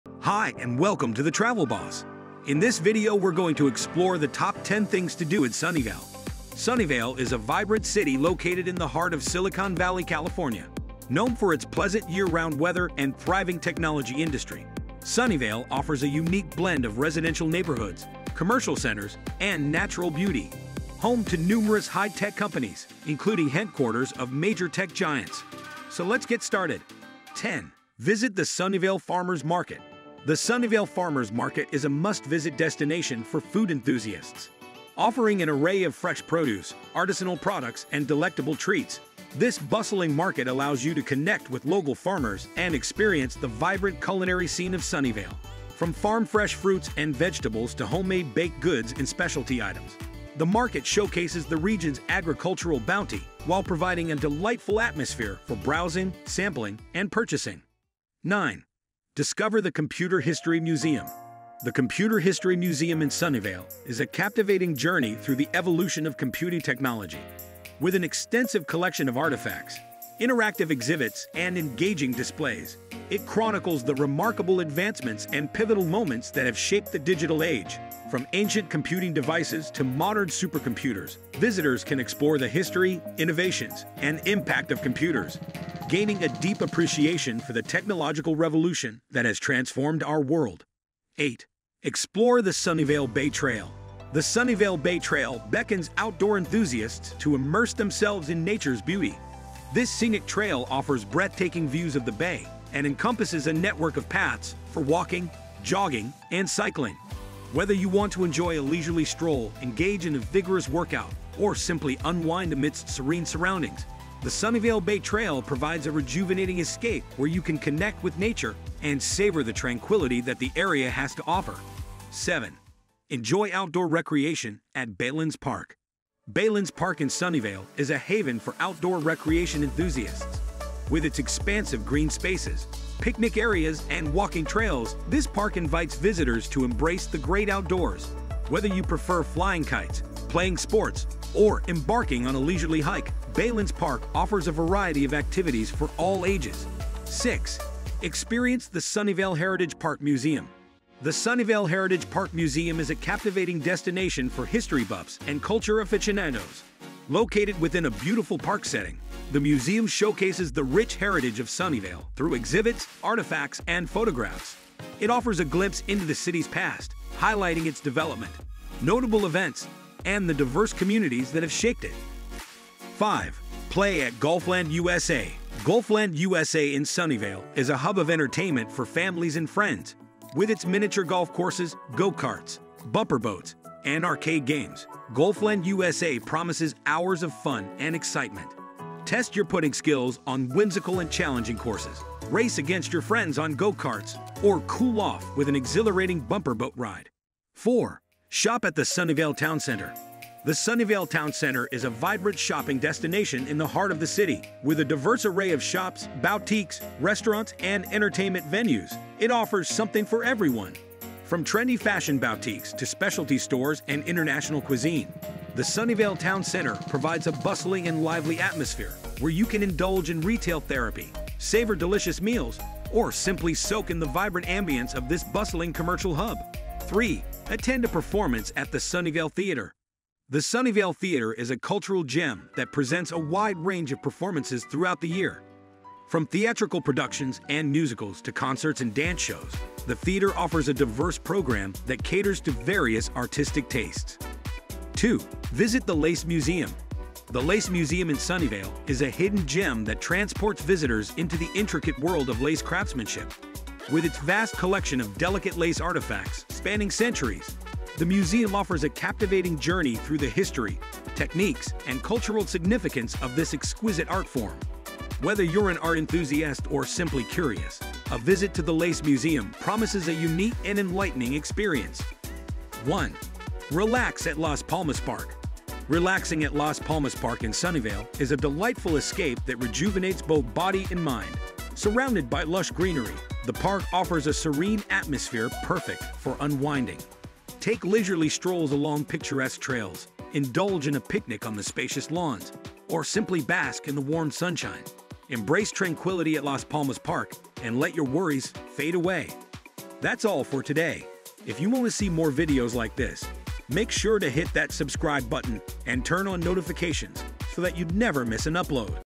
TOP 10 Things to do in Sunnyvale, California 2023!
Sunnyvale, California is an amazing place to visit in California. If you want to know top 10 what to do in Sunnyvale or you need a travel guide, please keep watching

5. Play at Golfland USA: Golfland USA in Sunnyvale is a hub of entertainment for families and friends. With its miniature golf courses, go-karts, bumper boats, and arcade games, Golfland USA promises hours of fun and excitement. Test your putting skills on whimsical and challenging courses, race against your friends on go-karts, or cool off with an exhilarating bumper boat ride.

4. Shop at the Sunnyvale Town Center: The Sunnyvale Town Center is a vibrant shopping destination in the heart of the city. With a diverse array of shops, boutiques, restaurants, and entertainment venues, it offers something for everyone. From trendy fashion boutiques to specialty stores and international cuisine, the Sunnyvale Town Center provides a bustling and lively atmosphere where you can indulge in retail therapy, savor delicious meals, or simply soak in the vibrant ambiance of this bustling commercial hub.

3. Attend a performance at the Sunnyvale Theatre: The Sunnyvale Theatre is a cultural gem that presents a wide range of performances throughout the year. From theatrical productions and musicals to concerts and dance shows, the theater offers a diverse program that caters to various artistic tastes.

2. Visit the Lace Museum: The Lace Museum in Sunnyvale is a hidden gem that transports visitors into the intricate world of lace craftsmanship. With its vast collection of delicate lace artifacts spanning centuries, the museum offers a captivating journey through the history, techniques, and cultural significance of this exquisite art form. Whether you're an art enthusiast or simply curious, a visit to the Lace Museum promises a unique and enlightening experience.

1. Relax at Las Palmas Park: Relaxing at Las Palmas Park in Sunnyvale is a delightful escape that rejuvenates both body and mind. Surrounded by lush greenery, the park offers a serene atmosphere perfect for unwinding. Take leisurely strolls along picturesque trails, indulge in a picnic on the spacious lawns, or simply bask in the warm sunshine. Embrace tranquility at Las Palmas Park and let your worries fade away.